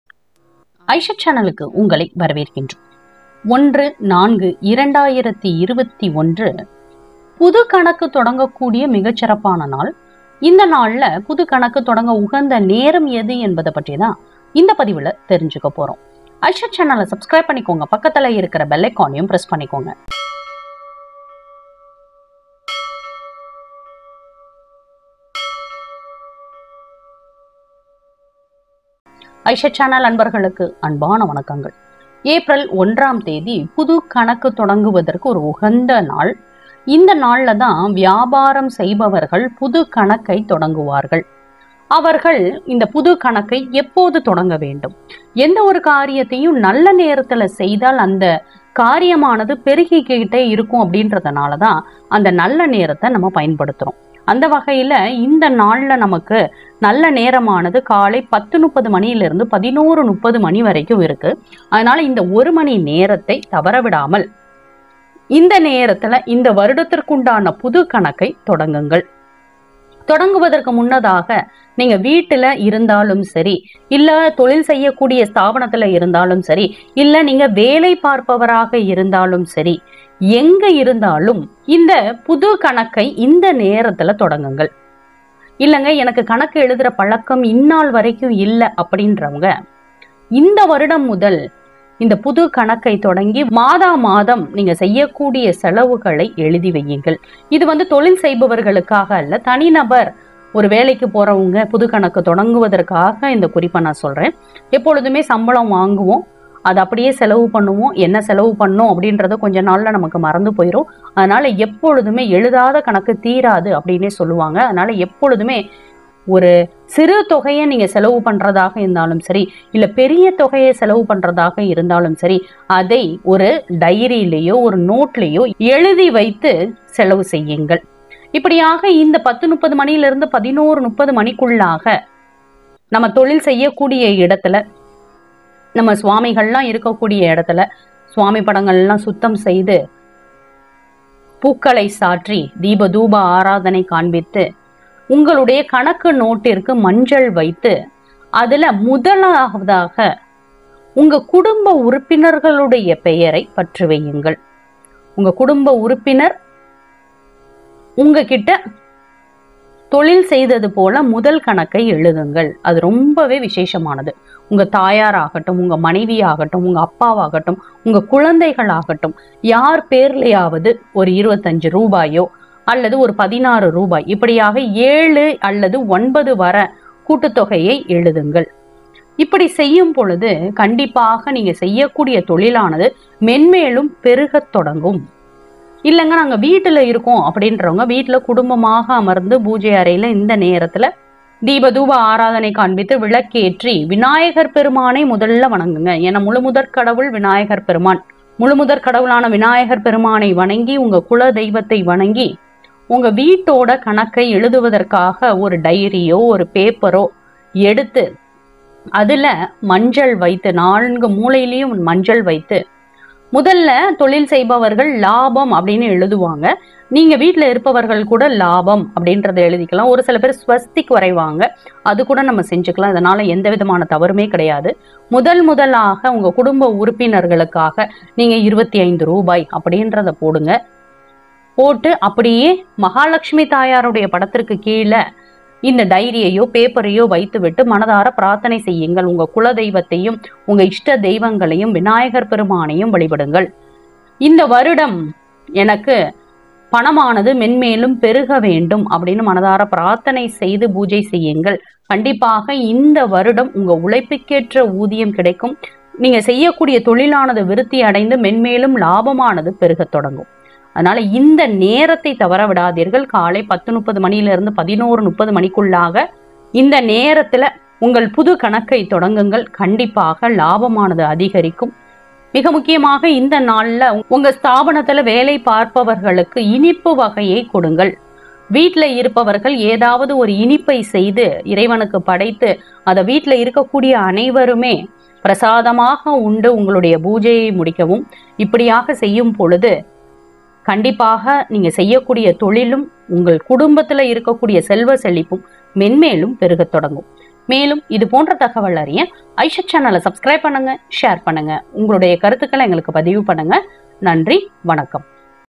01.04.2021 | Time to start new account |புது கணக்கு தொடங்க உகந்த நேரம்…!!!
நாளை ஏப்ரல் முதல் தேதி நிறுவனங்கள் புது கணக்கு ஆரம்பிக்க உகந்த நேரம் எது?

Keep this 3 things in north-east in your kitchen | சமையலறையில் இந்த 3 பொருளை சேர்த்து வைத்தால்